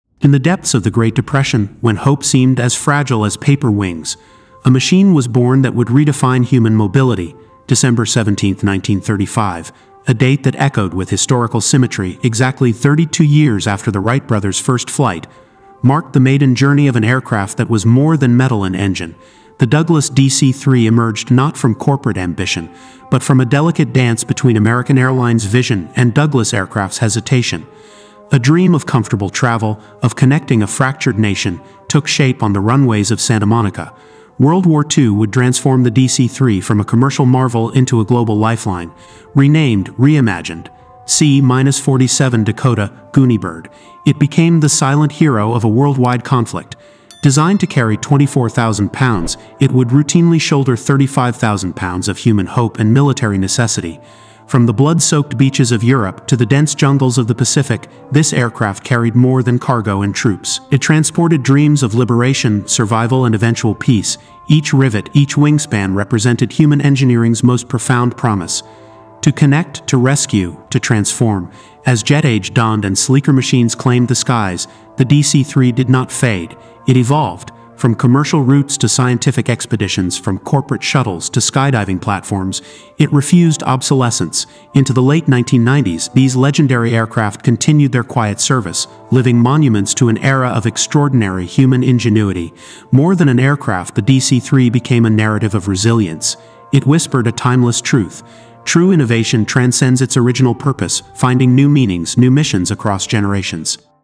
The DC-3's Extraordinary Voyage Starts - December 17, 1935
Explore the extraordinary journey of the Douglas DC-3, an aircraft that emerged during the Great Depression and became a global symbol of human adaptability and technological innovation. More than a mechanical marvel, this documentary reveals how a single aircraft design can embody the complex narrative of mid-20th century human aspiration.

From commercial aviation's early days through the tumultuous landscapes of World War II and into the jet age, the DC-3 represents a profound testament to engineering resilience and the ever-evolving relationship between technology and human dreams.